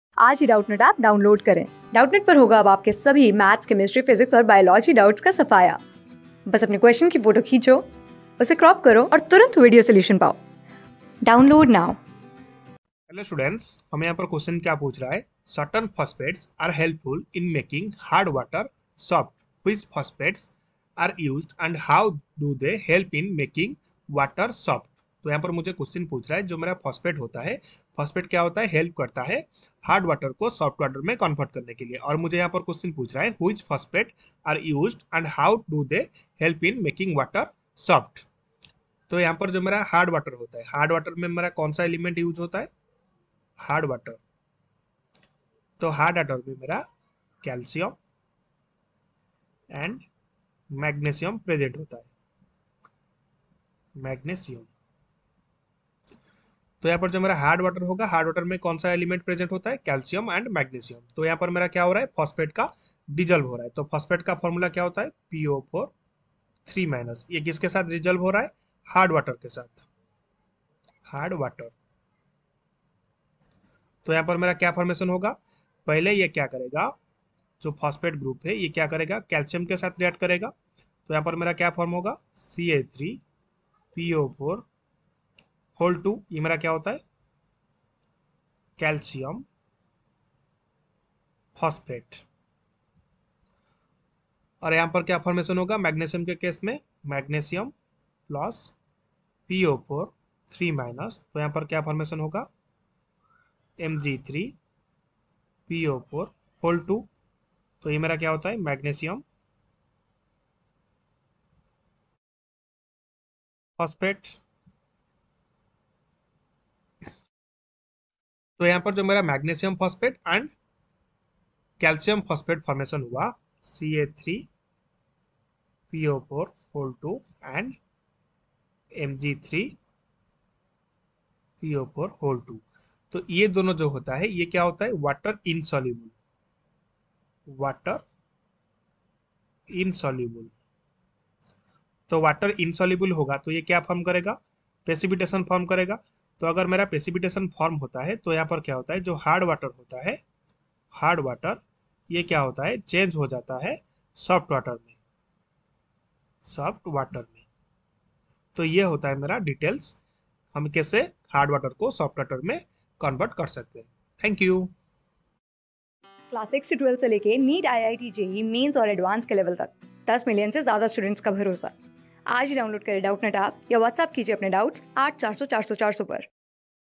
Certain phosphates are helpful in making hard water soft.Which phosphates are used and how do t...
Certain phosphates are helpful in making hard water soft.Which phosphates are used and how do they help in making water soft?
Class: 8
Subject: CHEMISTRY
Chapter: SOME IMPORTANT ELEMENTS AND THEIR COMPOUNDS
Board:FOUNDATION

👉 Have Any Query? Ask Us.